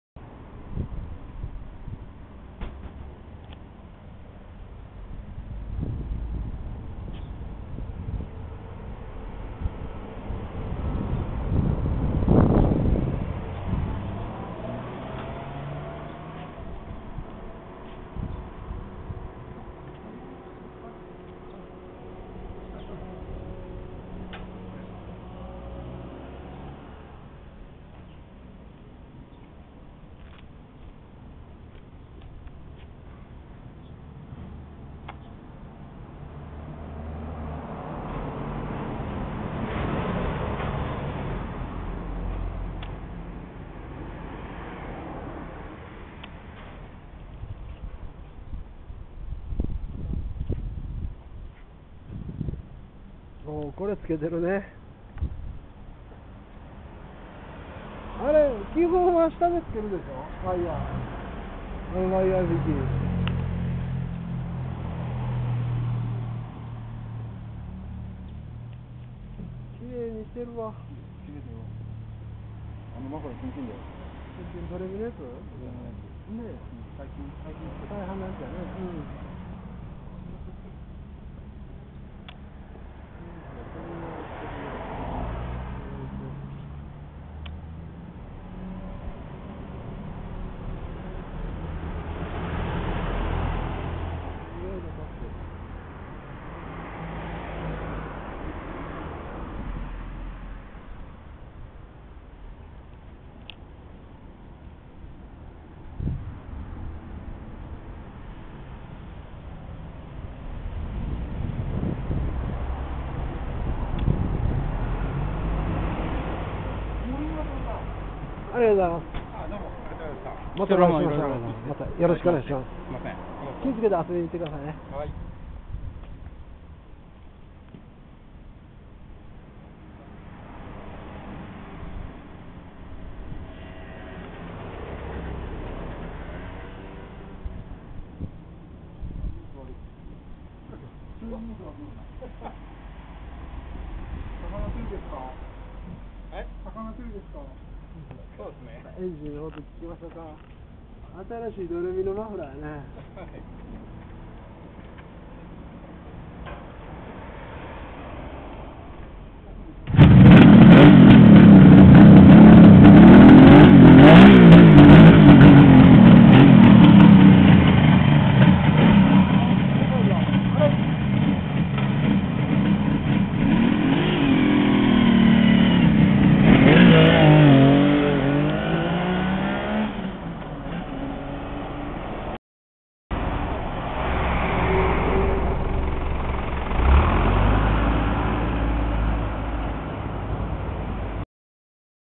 H1B 500SS MACHⅢ KAWASAKI 1972
名古屋から白浜向けて＾＾途中堺美原に寄って頂きました、

以前、従弟の結婚式のために臨時休業した時に来られたそうです、

さて出発です、

楽しんできてね